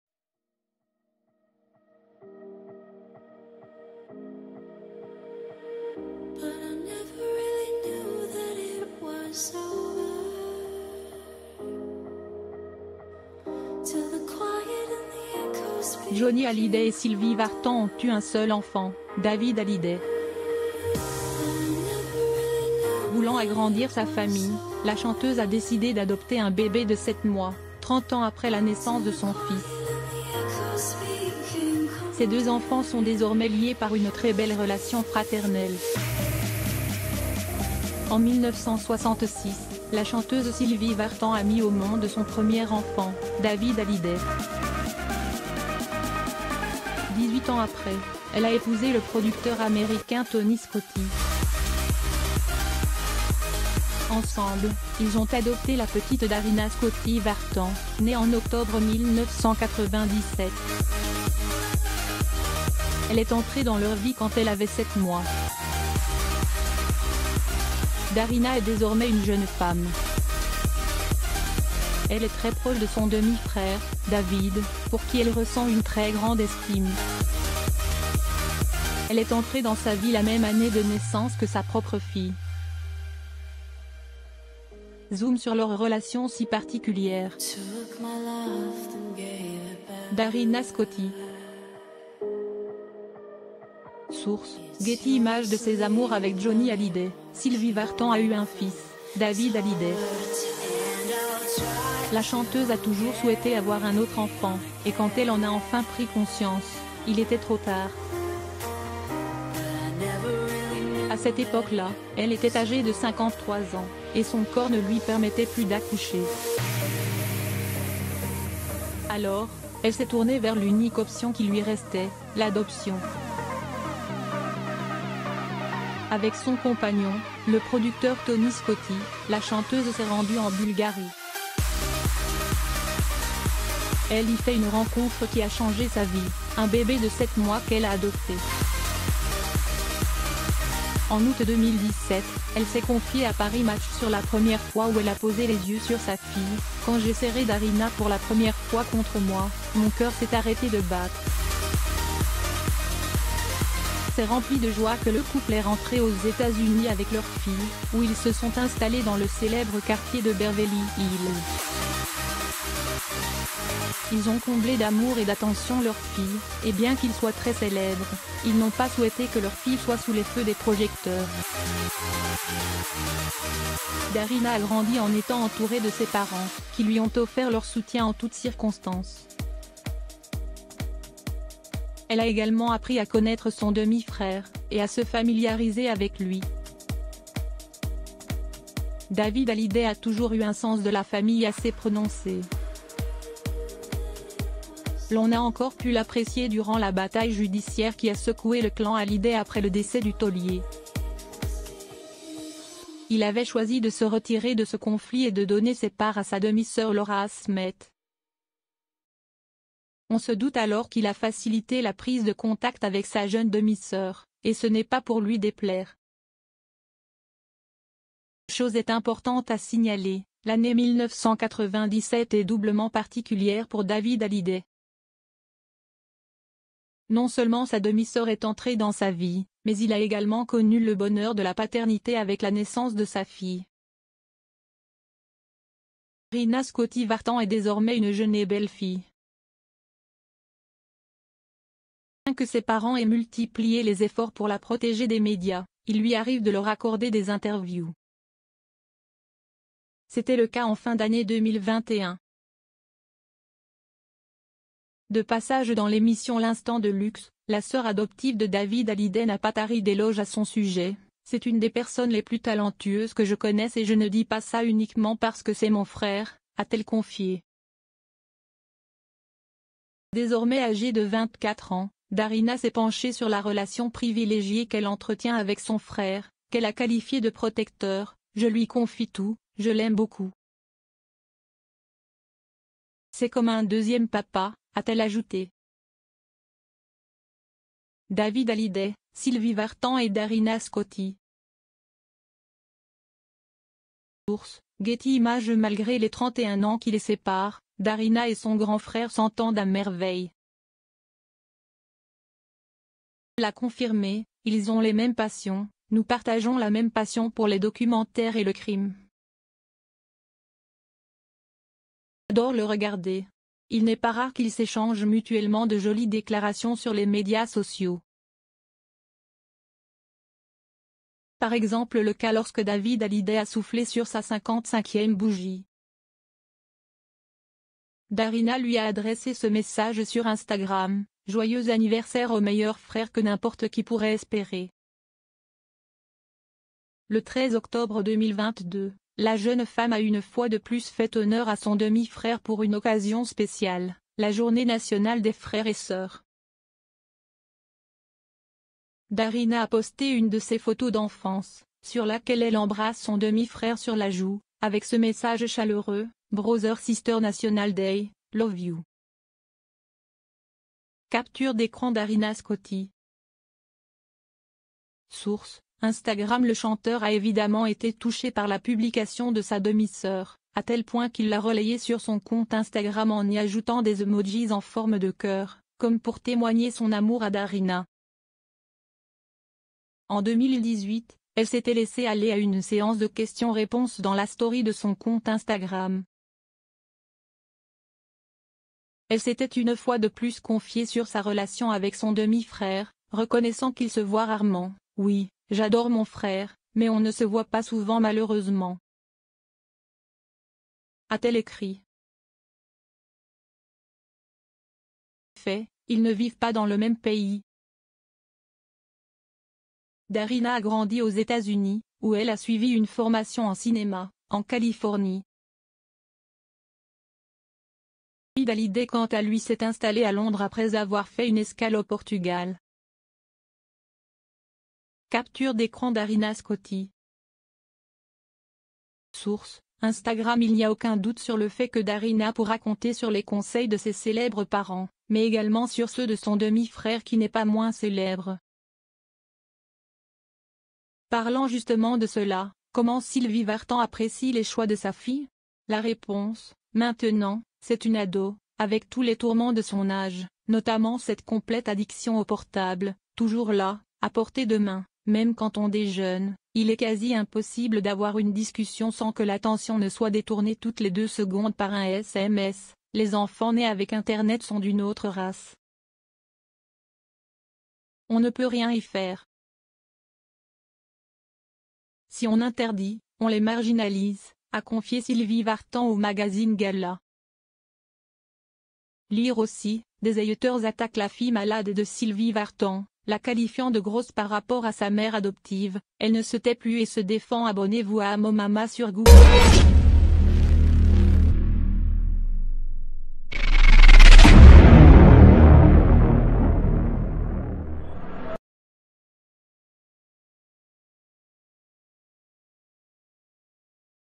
David Hallyday a accueilli sa fille Emma et sa demi-sœur Darina la même année - Leurs 31 ans d'éca
David Hallyday a accueilli sa fille Emma et sa demi-sœur Darina la même année - Leurs 31 ans d'éca Thank BUY Johnny Hallyday et Sylvie Vartan ont eu un seul enfant : David Hallyday. Voulant agrandir sa famille, la chanteuse a décidé d’adopter un bébé de 7 mois, 30 ans après la naissance de son fils. Ses deux enfants sont désormais liés par une très belle relation fraternelle. En 1966, la chanteuse Sylvie Vartan a mis au monde son premier enfant : David Hallyday. Dix-huit ans après, elle a épousé le producteur américain Tony Scotti. Ensemble, ils ont adopté la petite Darina Scotti-Vartan, née en octobre 1997. Elle est entrée dans leur vie quand elle avait 7 mois. Darina est désormais une jeune femme. Elle est très proche de son demi-frère, David, pour qui elle ressent une très grande estime. Elle est entrée dans sa vie la même année de naissance que sa propre fille. Zoom sur leur relation si particulière. Darina Scotti. І Source : Getty Images De ses amours ave